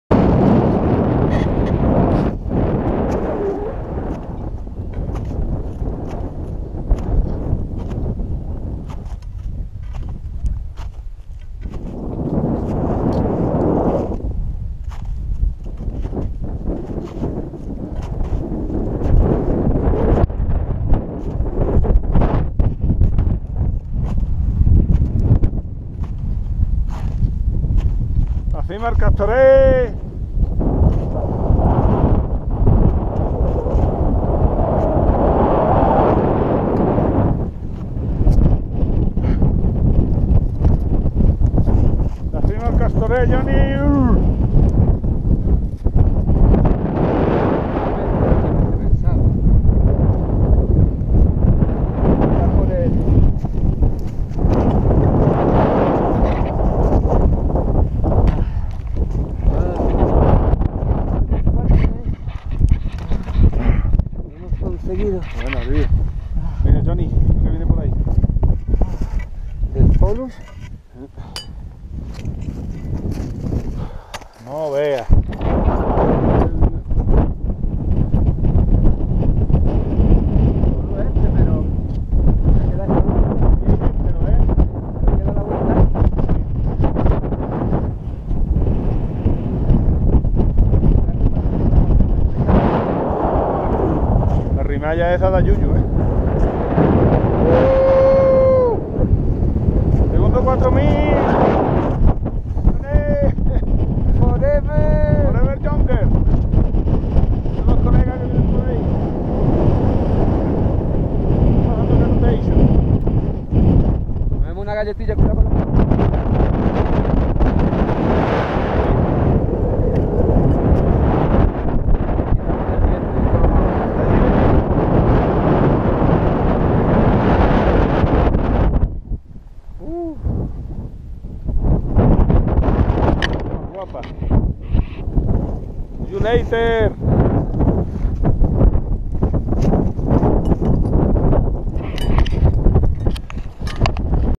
Llegando a la cima del Castor 4.228m - Monte Rosa
Llegada a la cima del Castor 4.228m. Monte Rosa, alpes italia. Desde el refugio Quintino Sella. Julio 2016